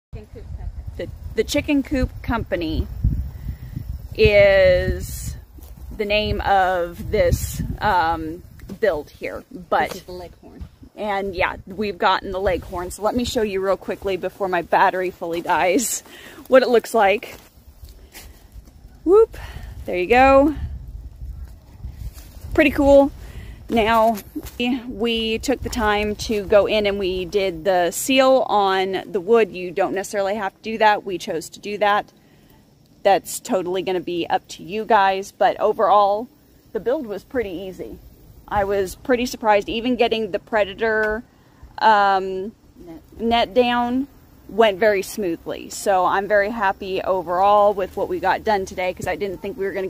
Thoughts On The Chicken Coop Company's Leghorn Chicken Coop Build
Here's my thoughts on the chicken coop company's leghorn chicken coop build! So far, we're loving it!